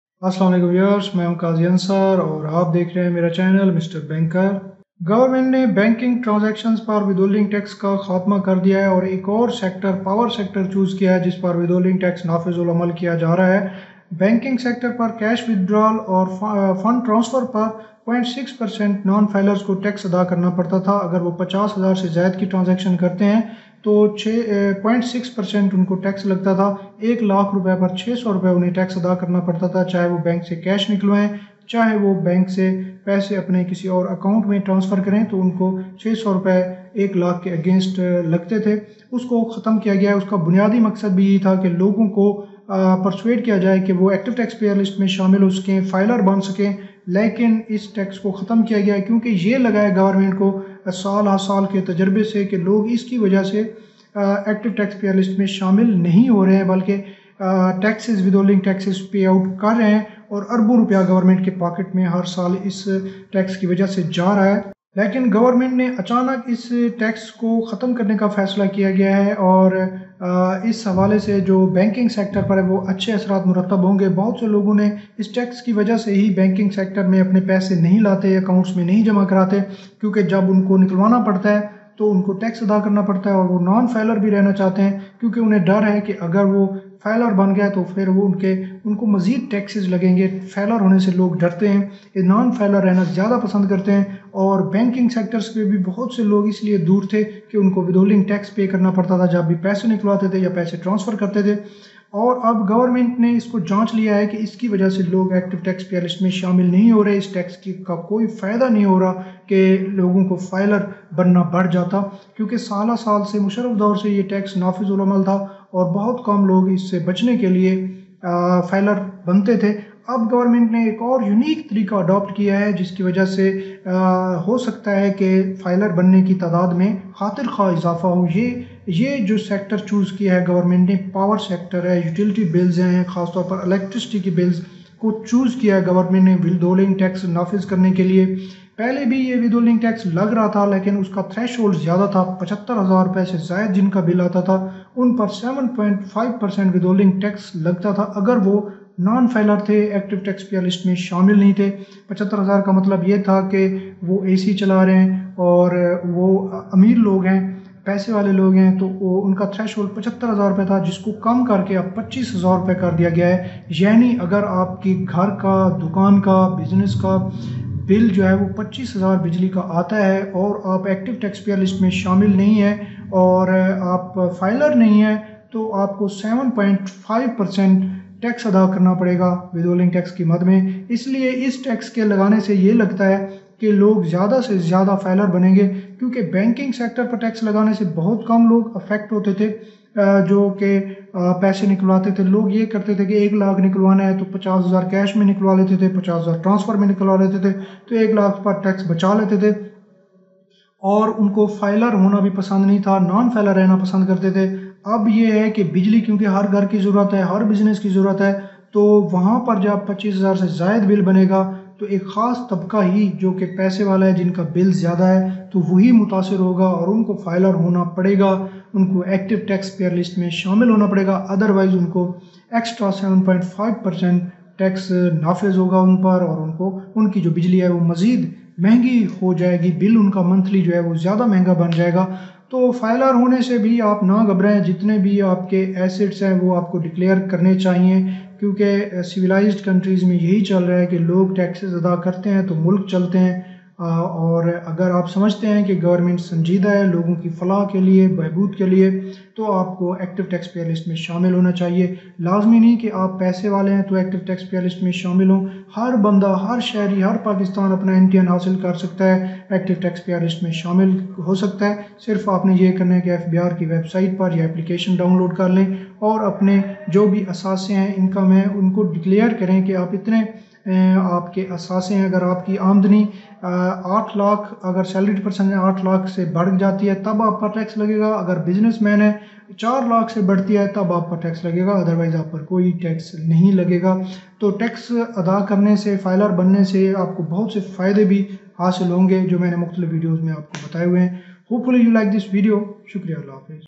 Non Filers to Pay WHT 7.5% on Electricity Bills
Govt has abolished Withholding tax on bank transactions for Non Filers but has introduced another unique tax on Electricity Bills for Non Filers.
Earlier for non Filer Threshold was Rs.75000 to impose a WHT of 7.5% now threshold has been reduced to Rs25000.
Now if any citizen's electricity bill crosses RS25000 he will be liable for a tax of Rs7.5%